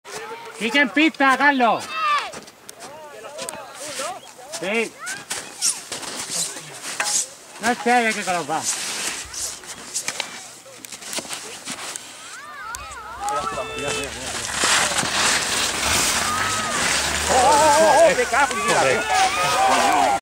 Skicross crash
Caida en los Campeonatos de Andalucia de Skicross